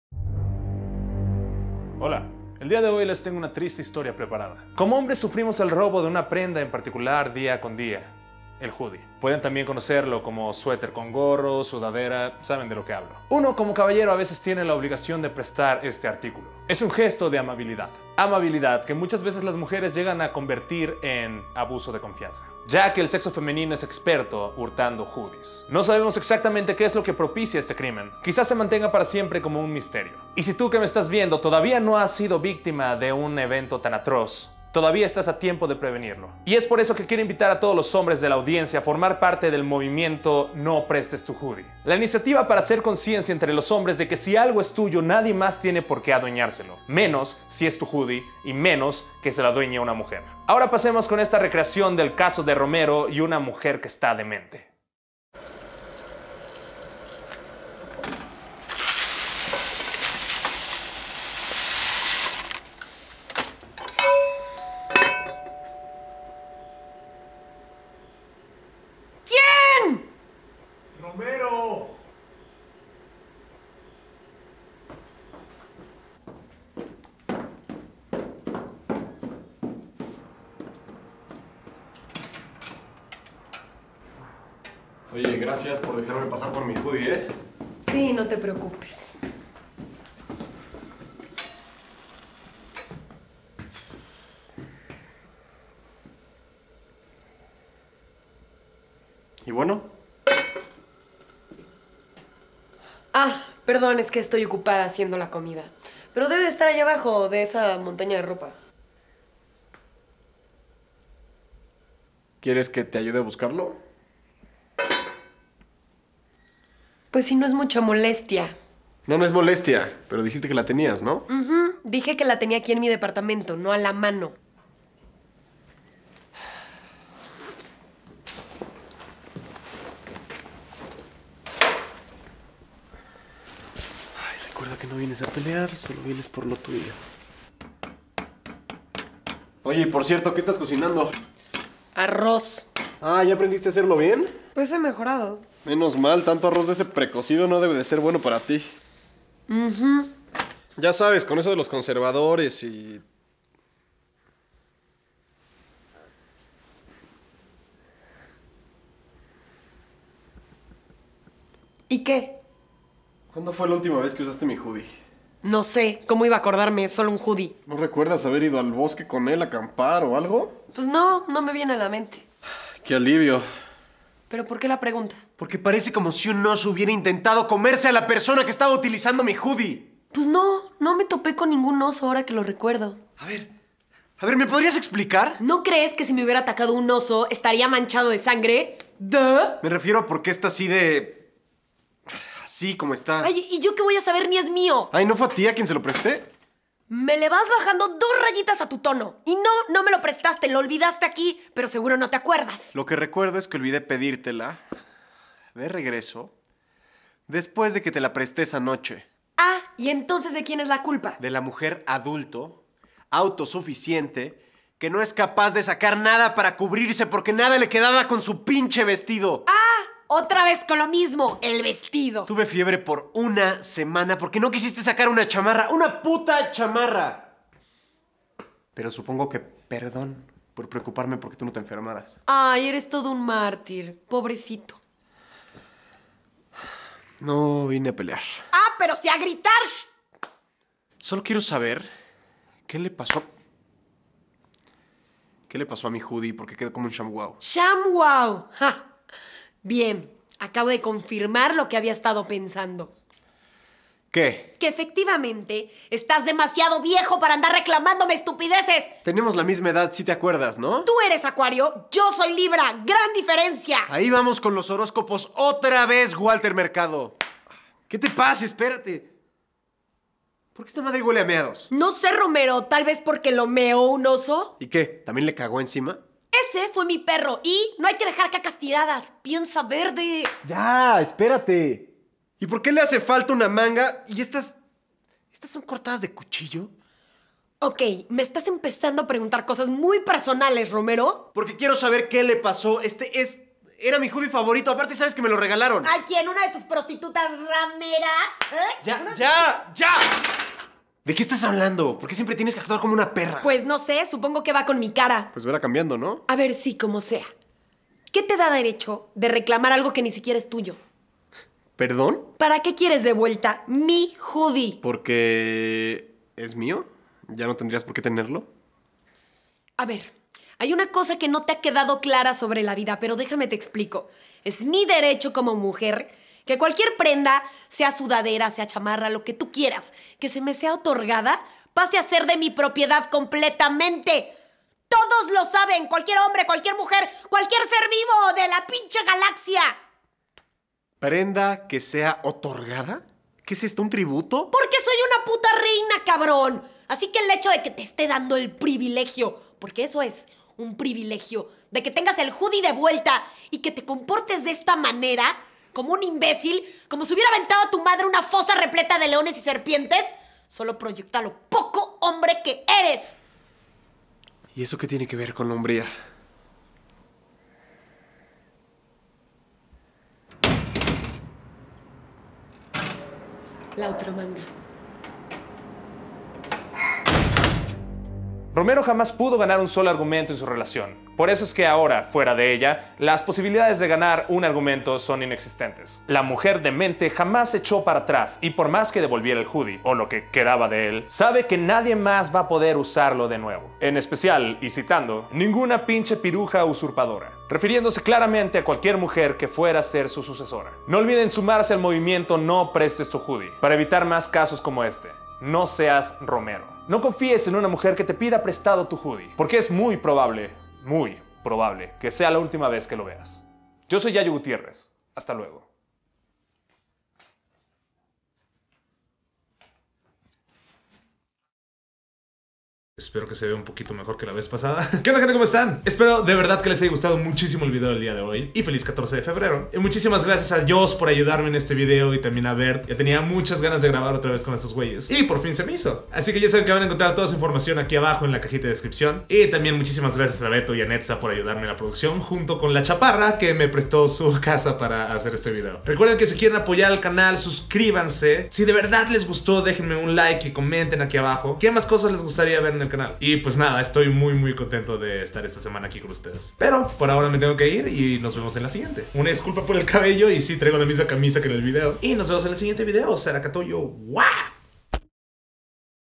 San Valengrinch: El Hoodie
Entonces se nos vino otro video de San Valengrinch después de... guau, creo que sí, años. Pero menos mal regresó. Espero que les haya gustado. Recuerden formar parte del movimiento NoPrestesTuHoodie para evitar más víctimas de esta terrible costumbre que tiene que ser erradicada de nuestra sociedad.

Les dejo aquí las redes de NoMeRevientes y de nuestros invitados por si quieren estar al pendiente de más contenido: